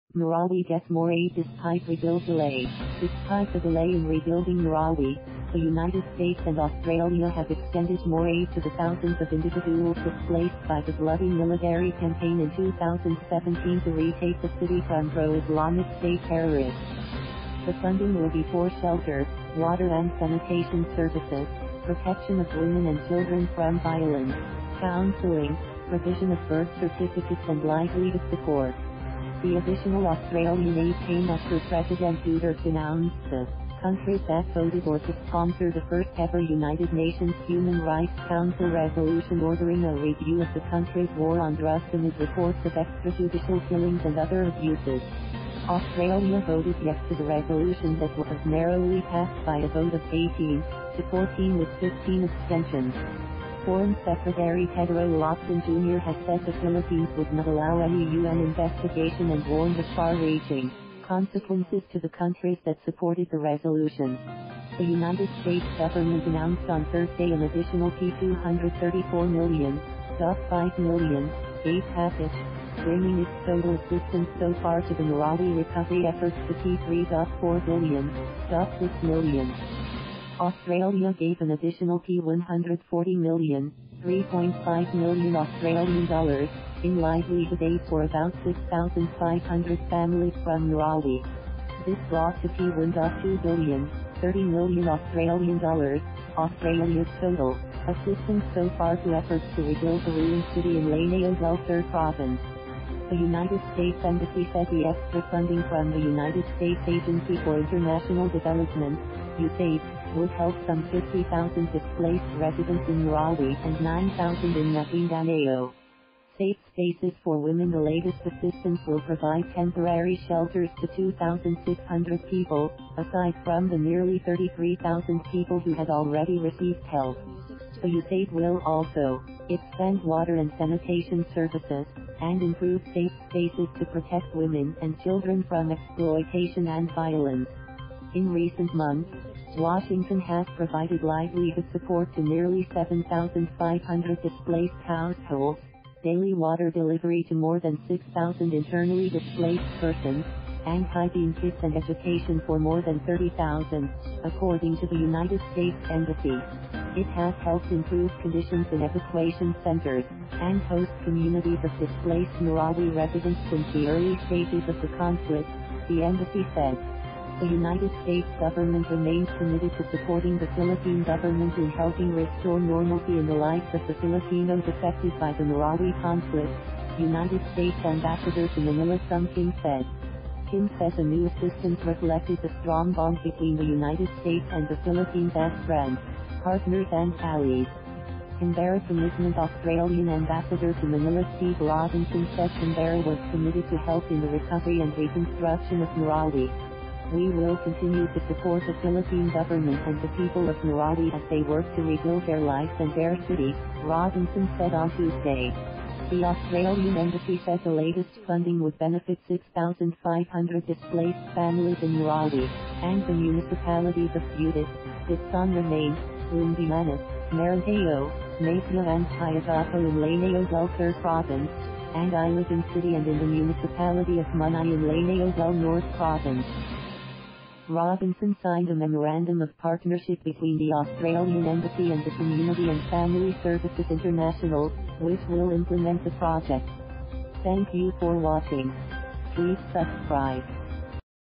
Marawi gets more aid despite rebuild delay
Despite the delay in rebuilding Marawi, the United States and Australia have extended more aid to the thousands of individuals displaced by the bloody military campaign in 2017 to retake the city from pro-Islamic State terrorists. The funding will be for shelter, water and sanitation services, protection of women and children from violence, counseling, provision of birth certificates and livelihood support. The additional Australian aid came after President Duterte denounced the countries that voted or cosponsored a first-ever United Nations Human Rights Council resolution ordering a review of the country’s war on drugs amid reports of extrajudicial killings and other abuses. Australia voted yes to the resolution that was narrowly passed by a vote of 18-14 with 15 abstentions. Foreign Secretary Teodoro Locsin Jr. has said the Philippines would not allow any UN investigation and warned of “far-reaching consequences” to the countries that supported the resolution. The US government announced on Thursday an additional P234 million ($4.5 million) aid package, bringing its total assistance so far to the Marawi recovery efforts to P3.4 billion ($63.6 million). Australia gave an additional P140 million (AUD 3.5 million) in livelihood aid for about 6,500 families from Marawi. This brought to P1.2 billion (AUD 30 million) Australia’s total assistance so far to efforts to rebuild the ruined city in Lanao del Sur province. The US Embassy said the extra funding from the US Agency for International Development (USAID) would help some 50,000 displaced residents in Marawi and 9,000 in Maguindanao. Safe spaces for women The latest assistance will provide temporary shelters to 2,600 people, aside from the nearly 33,000 people who had already received help. The USAID will also expand water and sanitation services, and improve safe spaces to protect women and children from exploitation and violence. In recent months, Washington has provided livelihood support to nearly 7,500 displaced households, daily water delivery to more than 6,000 internally displaced persons, and hygiene kits and education for more than 30,000, according to the US Embassy. It has helped improve conditions in evacuation centers and host communities of displaced Marawi residents since the early stages of the conflict, the embassy said. “The US government remains committed to supporting the Philippine government in helping restore normalcy in the lives of the Filipinos affected by the Marawi conflict,” US Ambassador to Manila Sung Kim said. Kim said the new assistance reflected the strong bond between the United States and the Philippines as friends, partners and allies. Canberra commitment Australian Ambassador to Manila Steve Robinson said Canberra was committed to help in the recovery and reconstruction of Marawi. “We will continue to support the Philippine government and the people of Marawi as they work to rebuild their lives and their city,” Robinson said on Tuesday. The Australian Embassy said the latest funding would benefit 6,500 displaced families in Marawi and the municipalities of Butig, Ditsaan-Ramain, Lumbayanague, Marantao, Masiu and Piagapo in Lanao del Sur province, and Iligan City and in the municipality of Munai in Lanao del Norte province.
Robinson signed a memorandum of partnership between the Australian Embassy and the Community and Family Services International, which will implement the project.